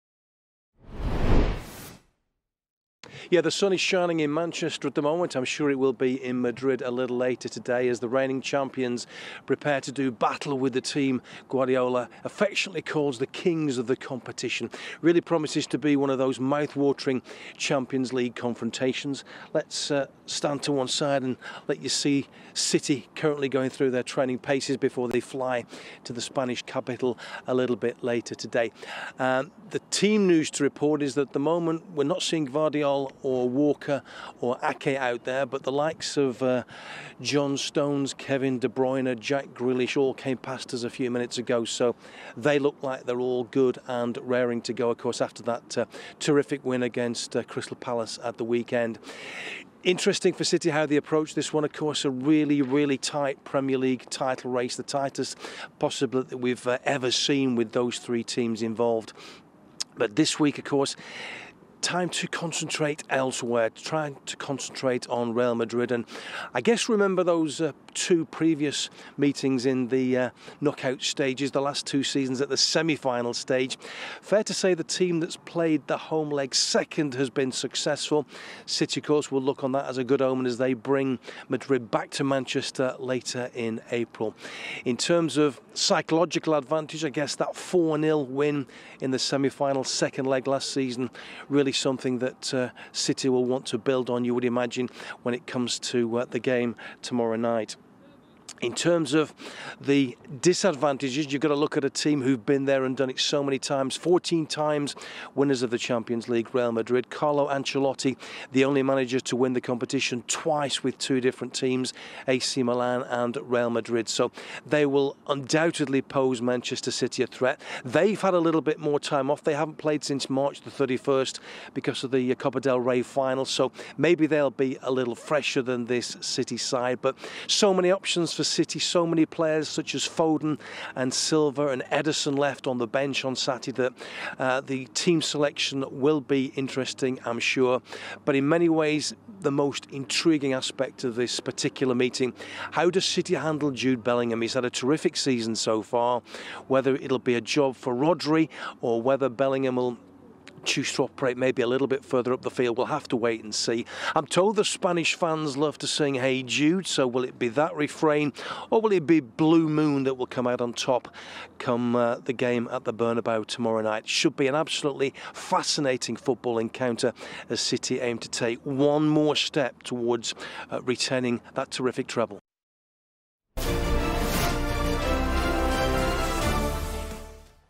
Man City train ahead of Real Madrid in the Champions League quarter-final
Watch Man City train ahead of Real Madrid in the Champions League quarter-final